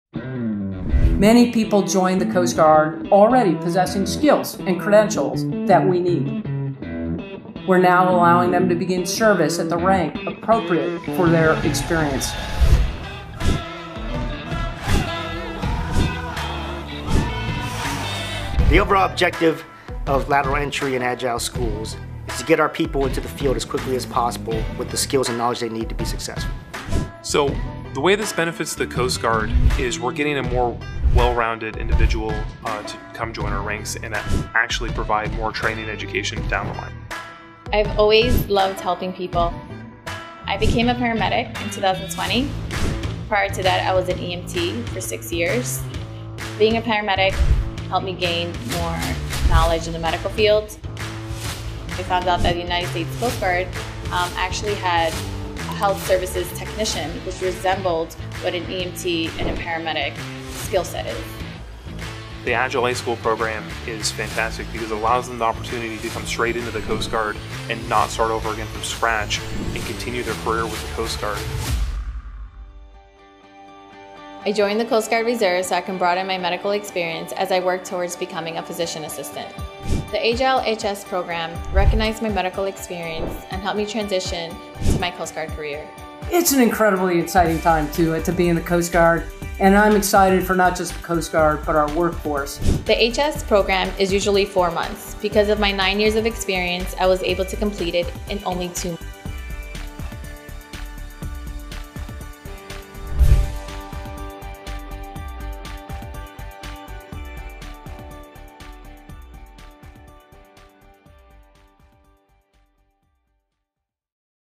The agile "A" school program
Petty Officer 3rd Class Monica Roman, a health services technician at the Coast Guard Sector New York clinic, discusses her experience with the Coast Guard's new Agile "A" School system.   "A" School, also written as A-School, are the Coast Guard's training schools for ratings.  Similar to the MOS training schools of other branches.

This new system allowed Roman to graduate sooner than most "A" school students due to her prior experience as paramedic and EMT.

The Coast Guard is using programs like Agile "A" School and Lateral Entry to help members quickly advance if they have prior experience and certified credentials to their field.

(U. S. Coast Guard Video by Petty Officer 1st Class Brandon Giles, Chief Warrant Officer Brandyn Hill, Petty Officer 3rd Class Ryan Schultz, Petty Officer 3rd Class Logan Kaczmarek, Petty Officer 3rd Class Taylor Bacon, Petty Officer 3rd Class Hunter Schnabel)